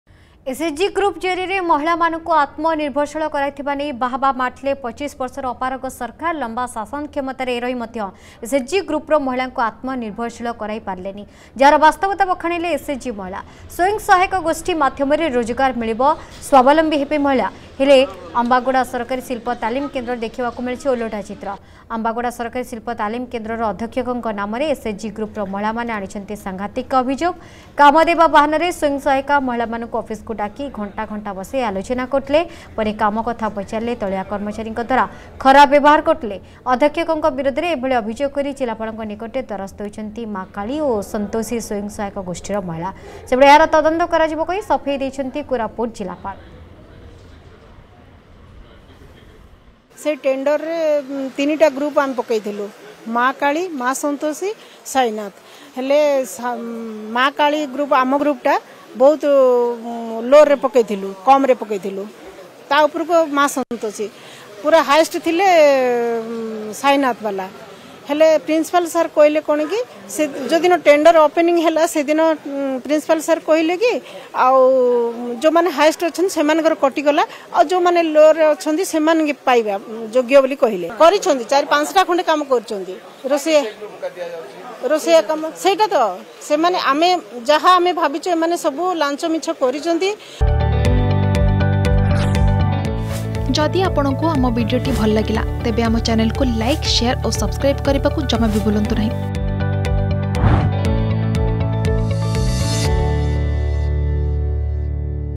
ବାସ୍ତବତା ବଖାଣିଲେ SHG ମହିଳା || SHG Groups Issue
ବାସ୍ତବତା ବଖାଣିଲେ SHG ମହିଳା । SHG ଗ୍ରୁପର ମହିଳାଙ୍କୁ ଆତ୍ମନିର୍ଭର ଶୀଳ କରାଇପାରିଲେନି । ମିଛ ପ୍ରହସନର ବାସ୍ତବତା ବଖାଣିଲେ SHG ମହିଳା । ତାଲିମ କେନ୍ଦ୍ରର ଅଧ୍ୟକ୍ଷ ଖରାପ ବ୍ୟବହାର କରୁଥିବା ଅଭିଯୋଗ ।